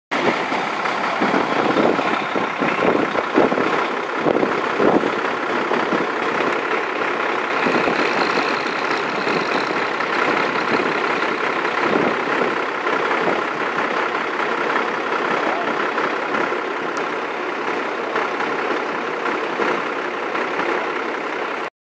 More from Beeac truck fire
Driving from Ballarat to Colac  and came across this little incident.